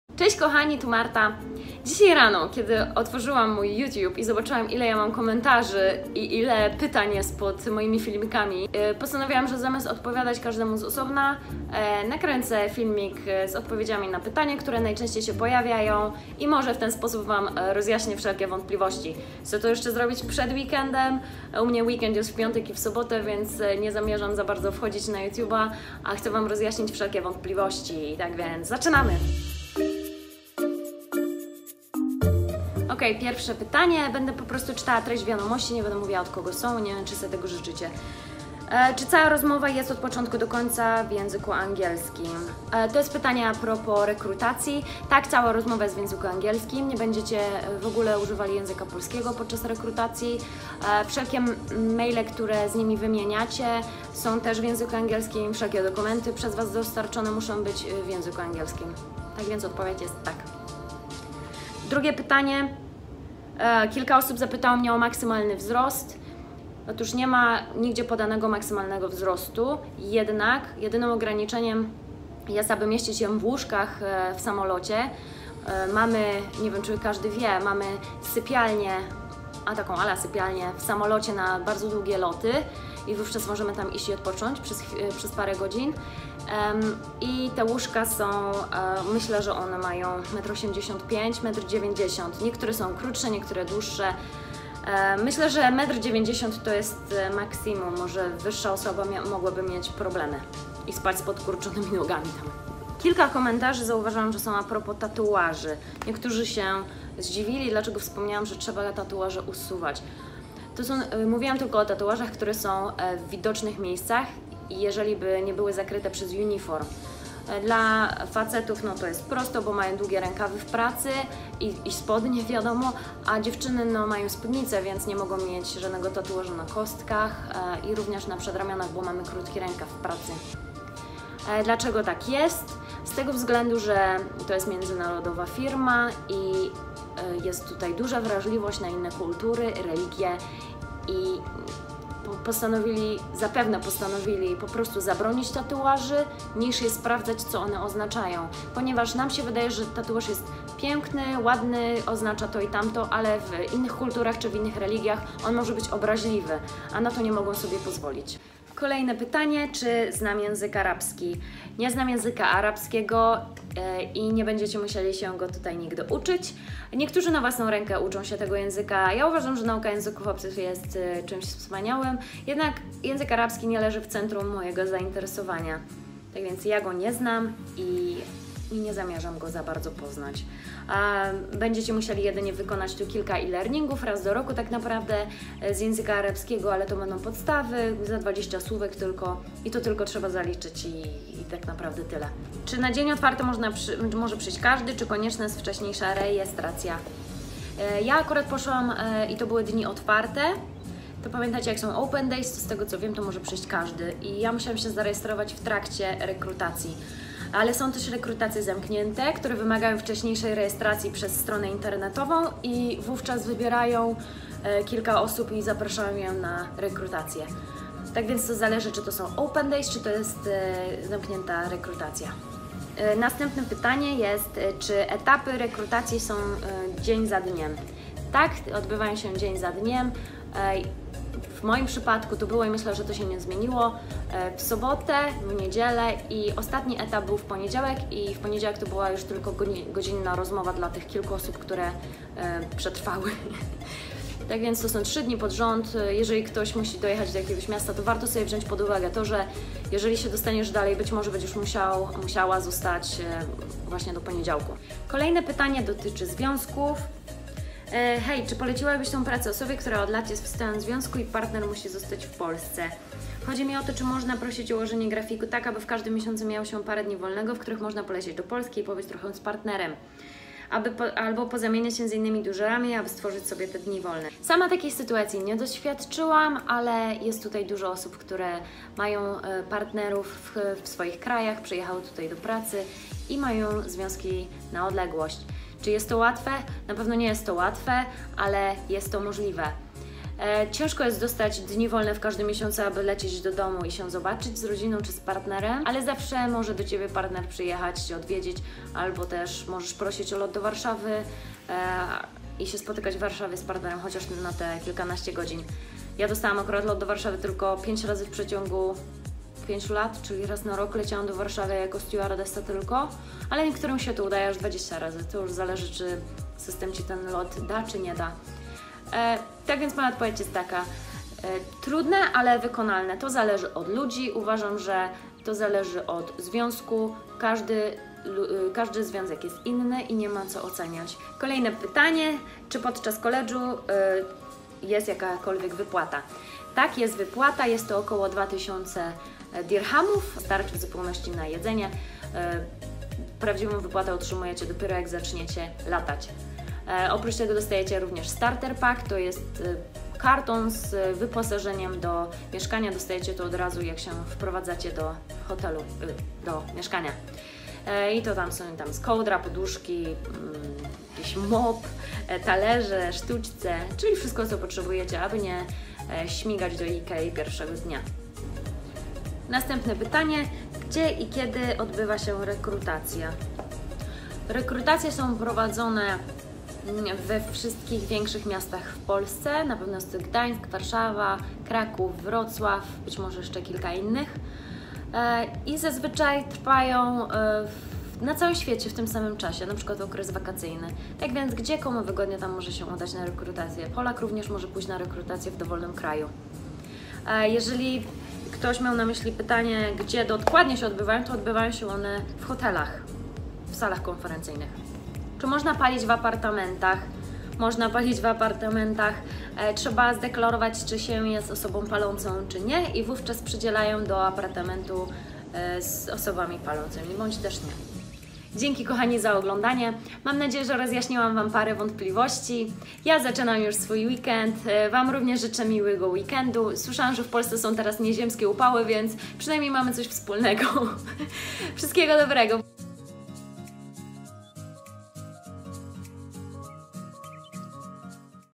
Pytania o Emirates: tatuaze, wyplata, zwiazki, jezyk arabski
Odpowiedzi na najczęściej zadawane przez Was pytania dotyczące pracy stewardess.

Zapraszam na moje inne vlogi

pozdrawiam
Marta